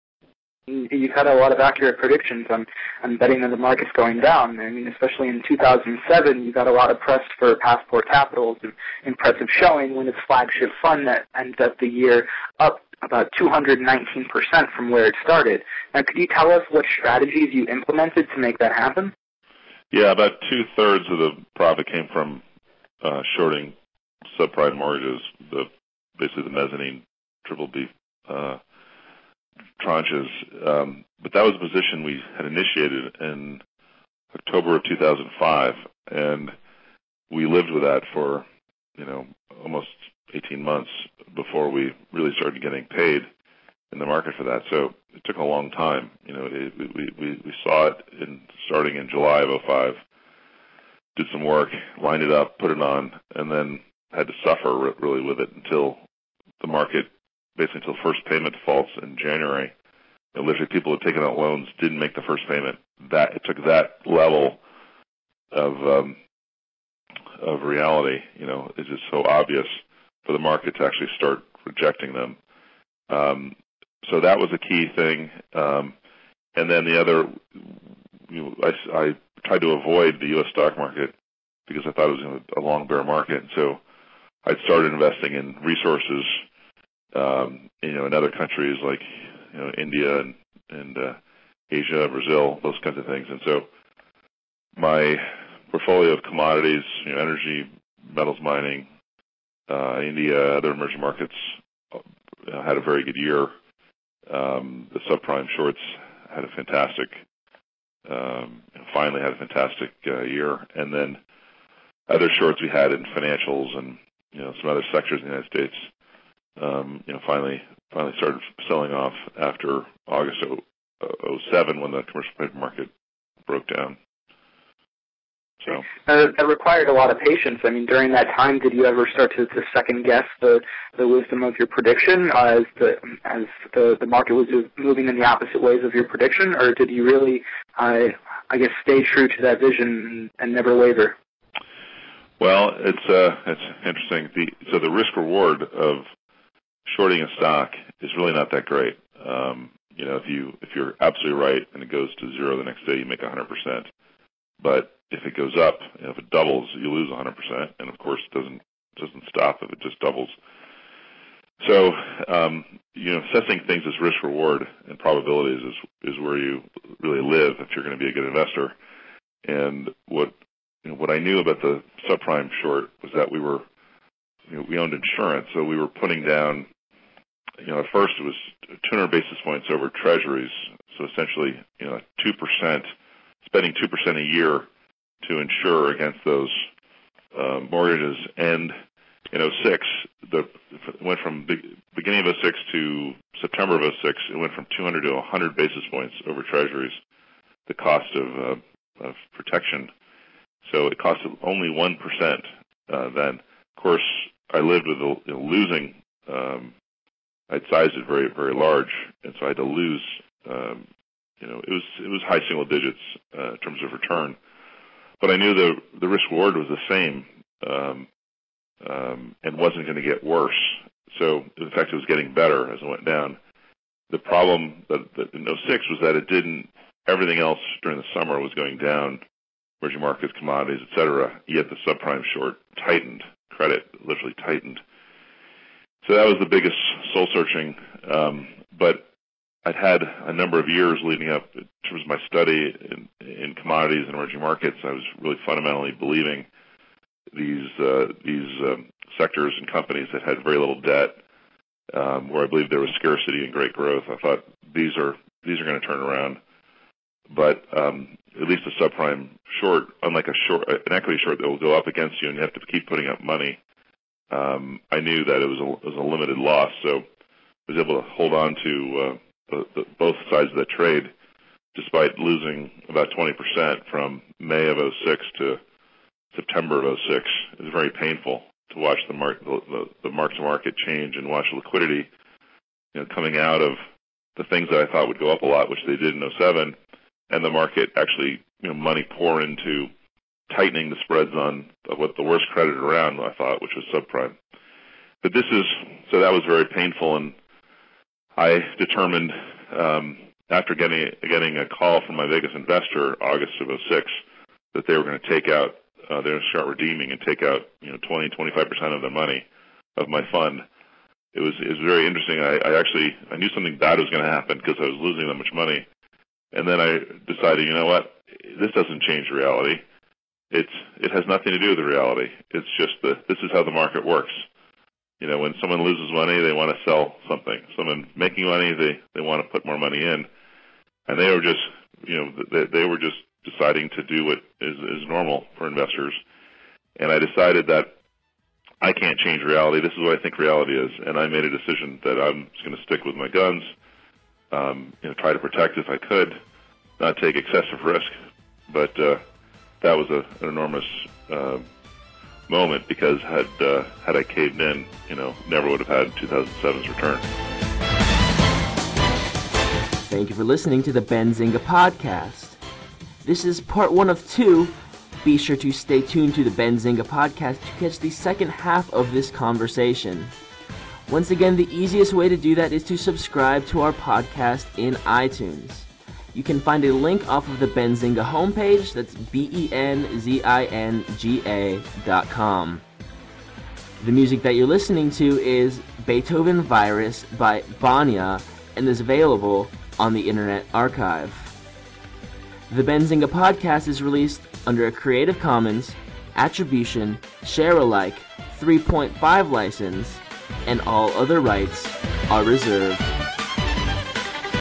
Pt. 2 A Passport to High Returns - Interview With John Burbank
John Burbank is the founder of the hedge fund Passport Capital. He is a widely followed money manager with a record of success since he launched his firm in 2000. John famously predicted the decline of the American housing market starting in 2005 and reaped a return of over 200% in 2007.